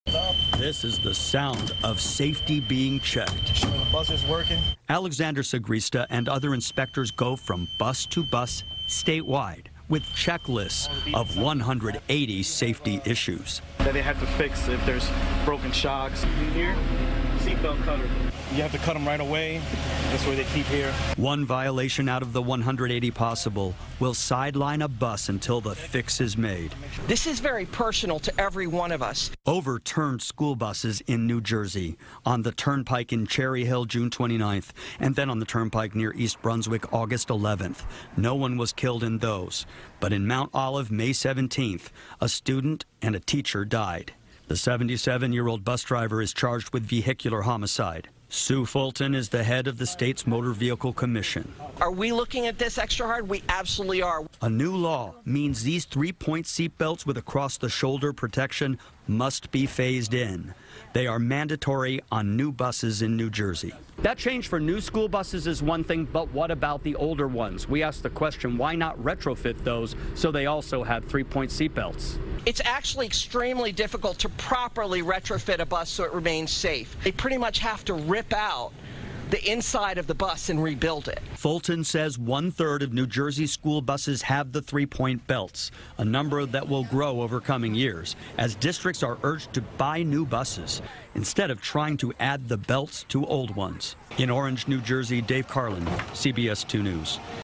New Jersey School Buses With Improved Seat Belts Inspected
New Jersey school buses with improved seat belts get inspected before the new school year, CBS2's Dave Carlin reports.